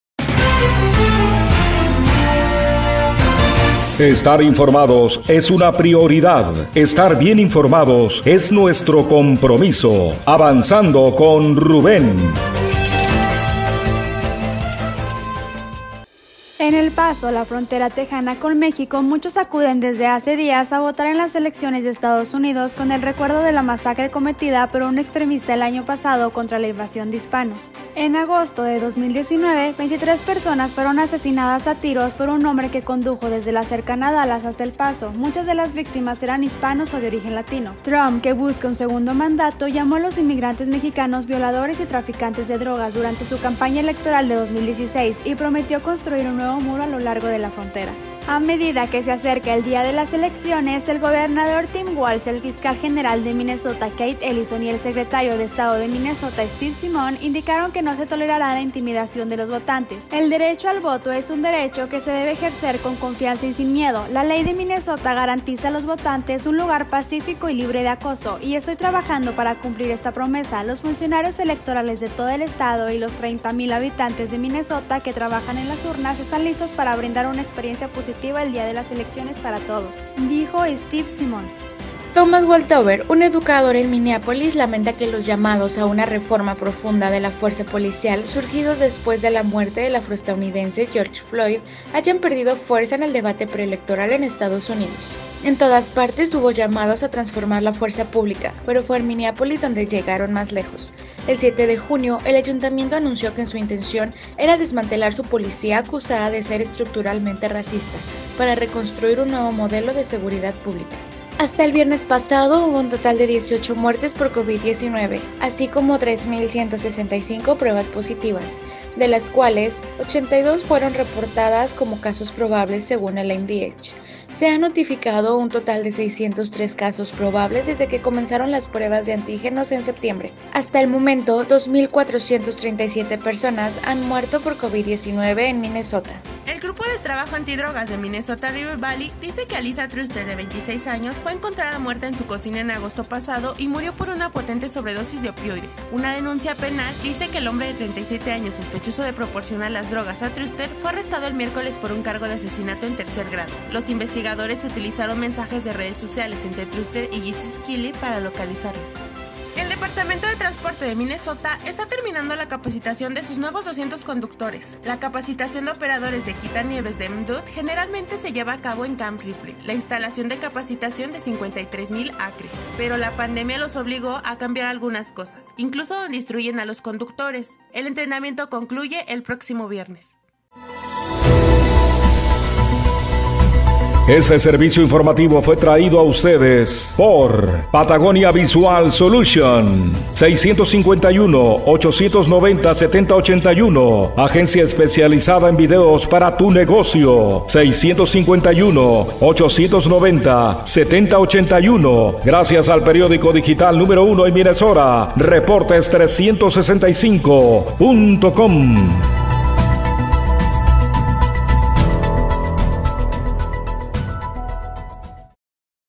En MINNEAPOLIS, y otras ciudades se SILENCIA la EXIGENCIA de REFORMA POLICIAL.
Noticias locales Minnesota, Estados Unidos